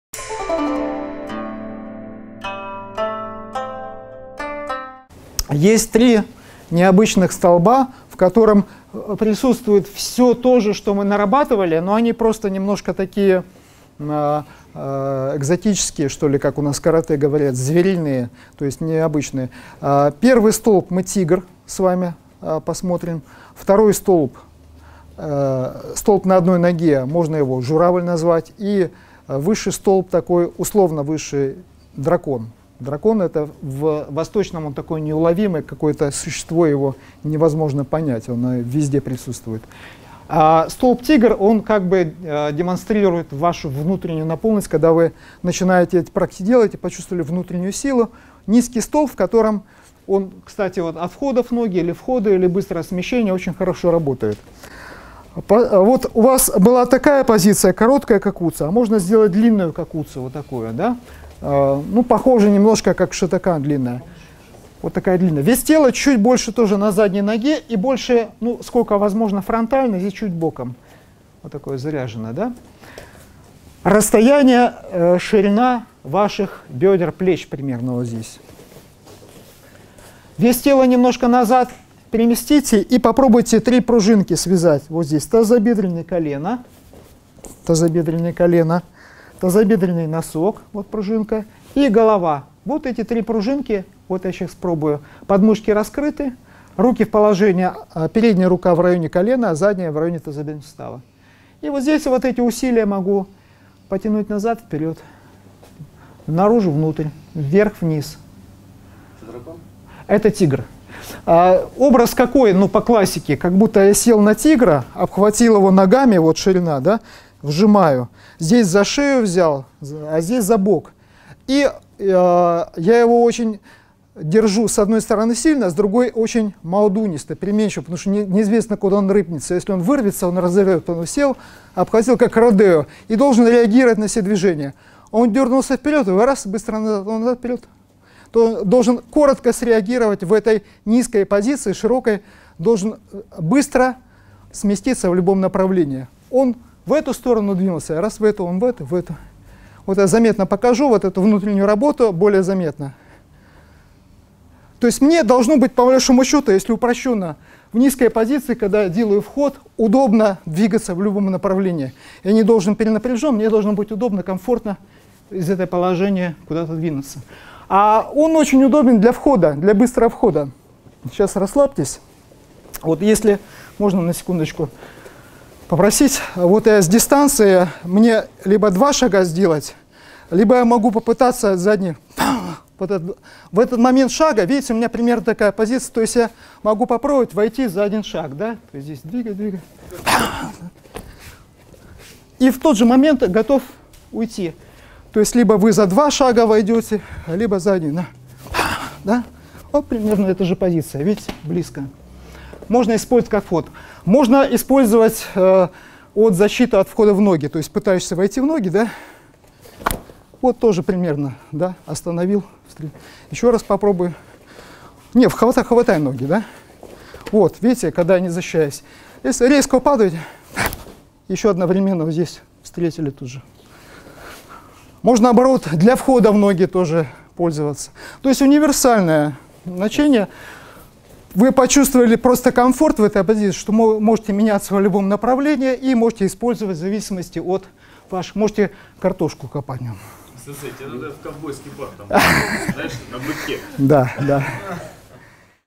#34 ИЦЮАНЬ-КЁКУСИНКАЙ
#34-й фильм из серии Ицюань-Кёкусинкай рассказывает о том, что такое "звериные столбы" и как усмирить Тигра.